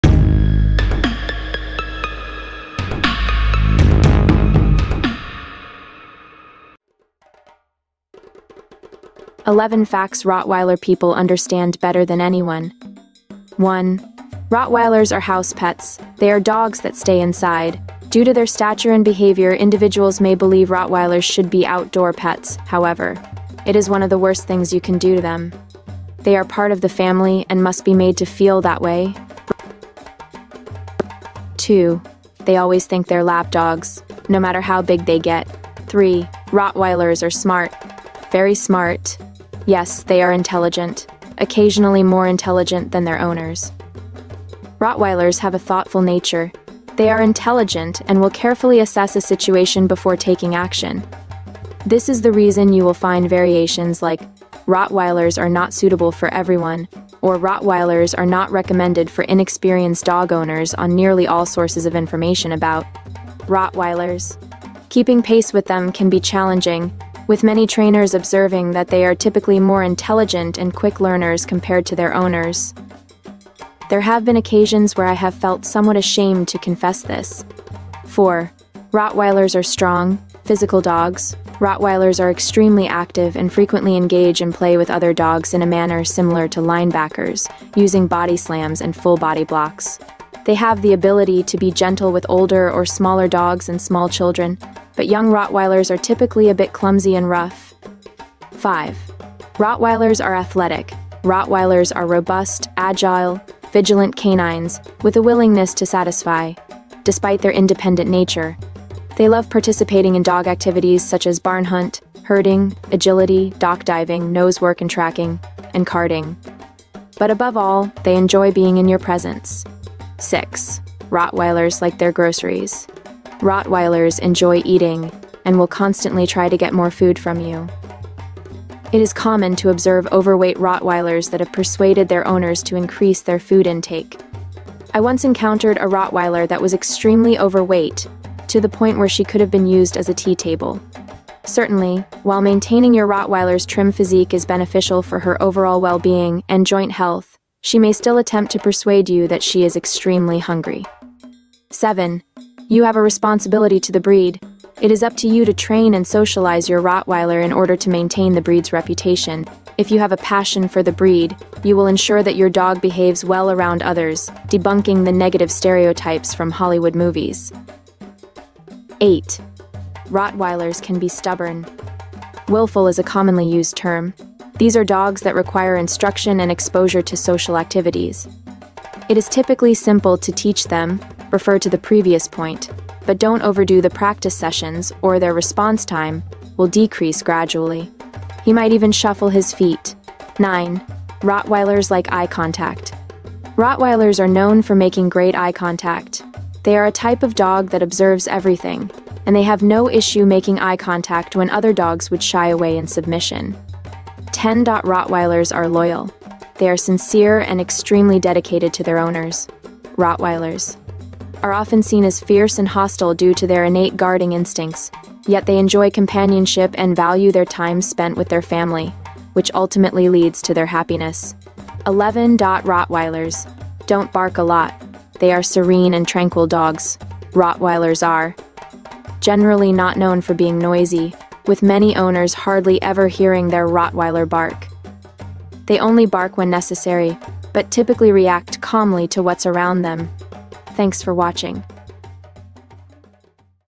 11 Facts Rottweiler People Understand Better Than Anyone
After being with Rottweilers for over thirty years, I sometimes feel like I am a
Rottweiler myself. In that period, I've cared for eight of my own pets and provided
temporary homes for dozens more through my involvement in breed rescue efforts.
Although each dog has its own unique personality - whether serious, playful, or social
- there are certain characteristics and information about Rottweilers that can
generally be anticipated.